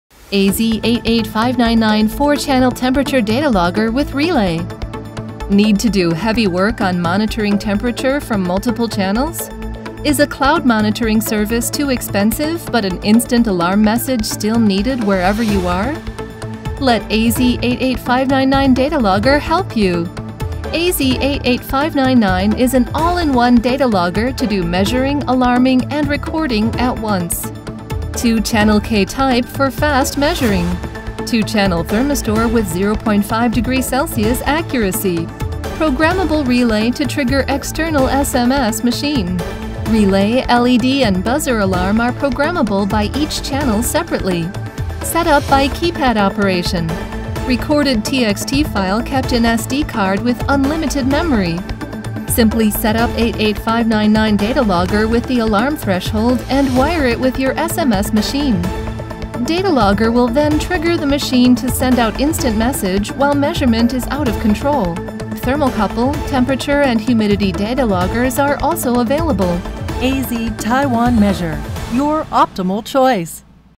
AZ 88599 4 channel temperature logger with SD card and relay
AZ's latest 4 channel temperature logger with SD card and relay. The relay can trigger external SMS machine to give instant alarm to your mobile phone any time.

♛ 88599 AZ 4 Channel K Thermocouple & Thermistor Thermal Thermometer Recorder with Relay features:
* Super big LCD portable / wall-mount SD card logger
* Measure and Log 4 temperature at once by using quick response K thermocouple and NTC Thermistor probes
* Programmable independent high/low alarm of each channel with relay output * One button press operation to display T1-T2
* Display temperature change from baseline
* Temperature offset function for adjustment purpose
* Relay to trigger external alarm system on (ex: GSM message)
* Hold function, Max/Min, offset, Low battery indicator
* Beeper and LED alarm are both available
* Programmable temperature High/Low alarm limit
* Over-range indication with error messages
* Blue back light function for dark area operation
* 12/24 hour time format selective
* Real time display (YEAR-MON-DATE, Hour-Min)
* 20 mins auto power off to save power
* Temperature units ℃/℉ selective